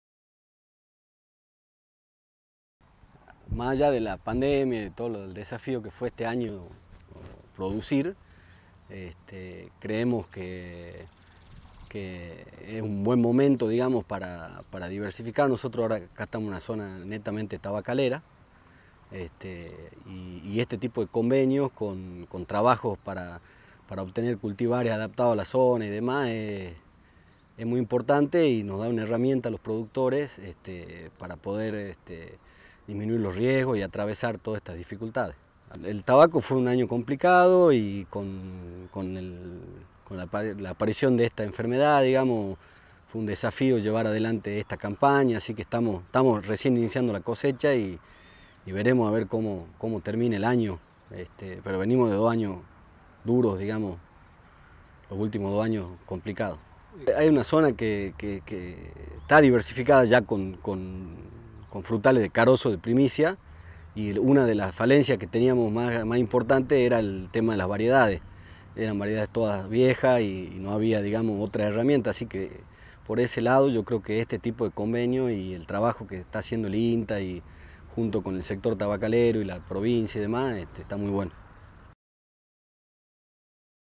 Frutales primicia , una alternativa rentable
Sebastian Marquez, productor tabacalero y miembro del Consejo Regional Salta - Jujuy, se refiere a la necesidad de diversificaciòn en el sector, en este caso con frutales de carozo - primicia.